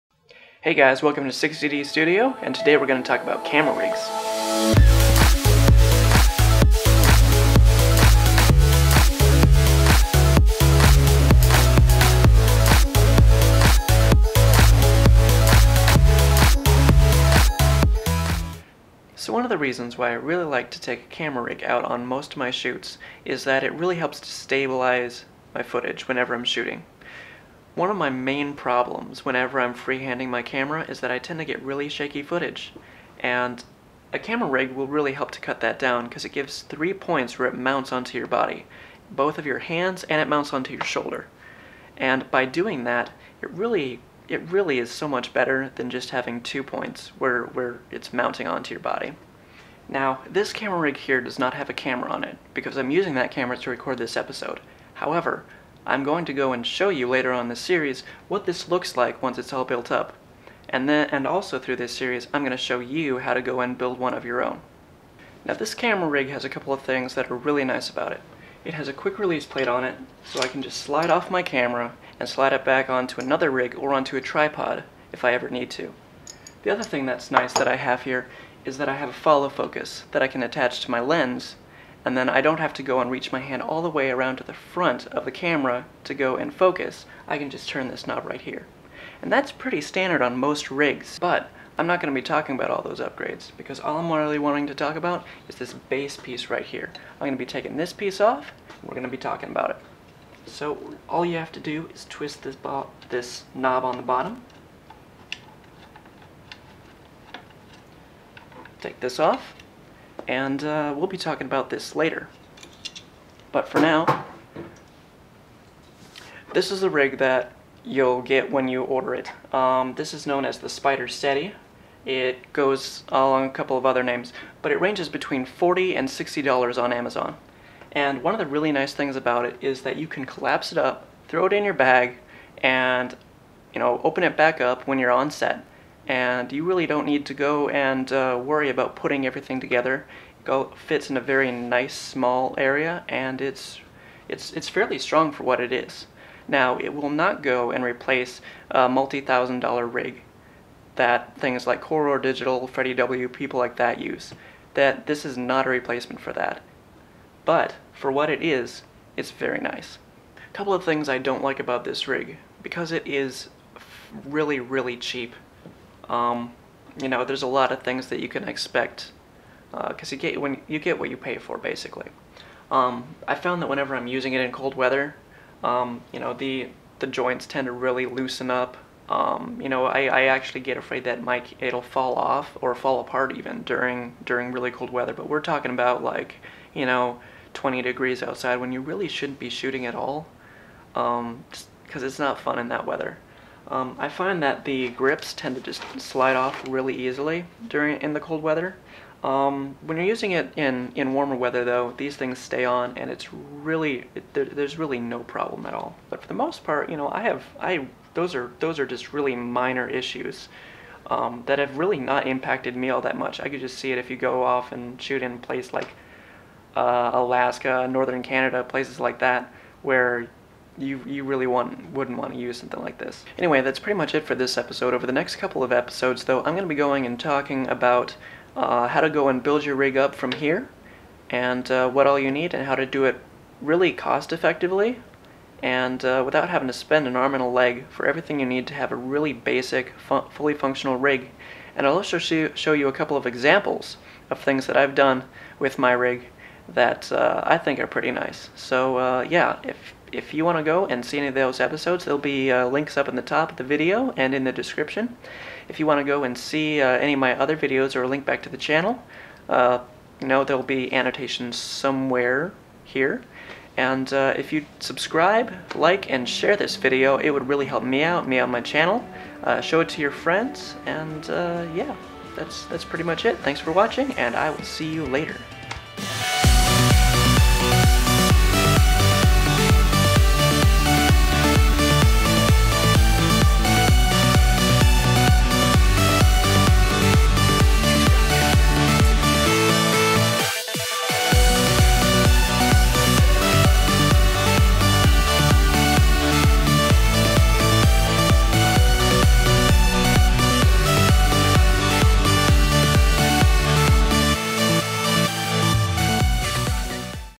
VDEO4Nubs | Episode 1 - Camera Rigs Part 1
This episode talks about Camera Rigs, and the basic benefits to having one, and introduces my low cost solution!
Links to products and music below!
Links to episodes will be posted in description as soon as they are made!
Spider Steady